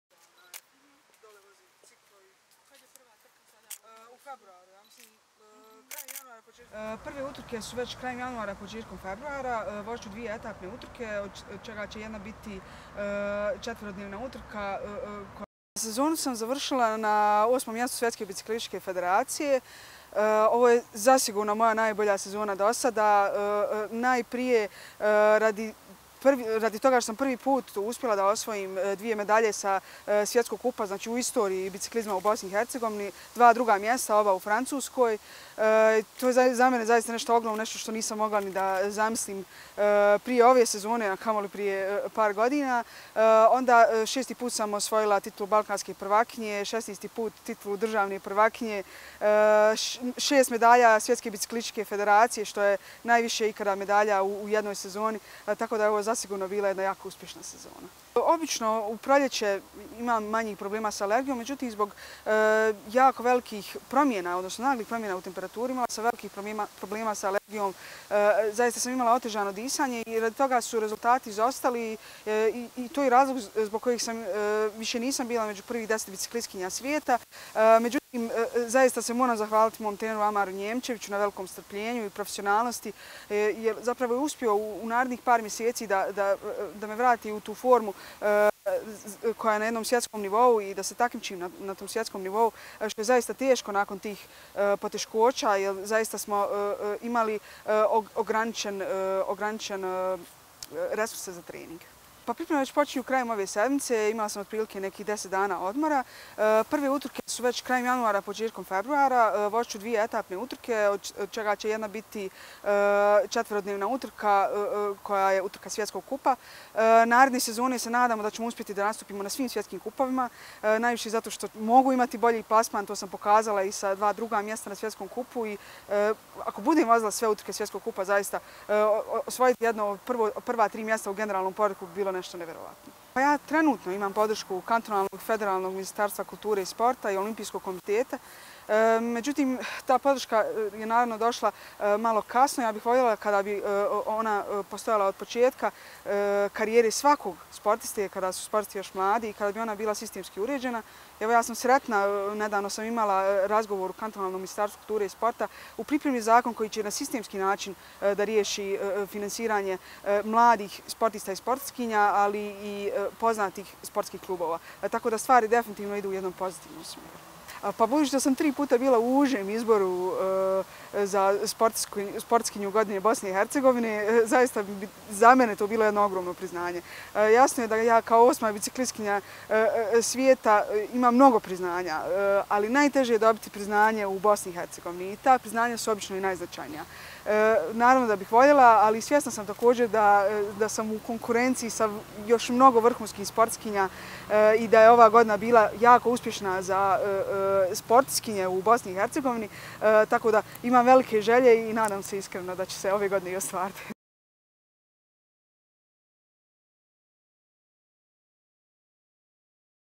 Tanović: Ovo je zasigurno moja najbolja sezona do sada, vikend
SARAJEVO, 24. oktobra (FENA) - Najbolja bosanskohercegovačka biciklistkinja Lejla Tanović završila je sezonu na osmom mjestu Svjetske biciklističke federacije.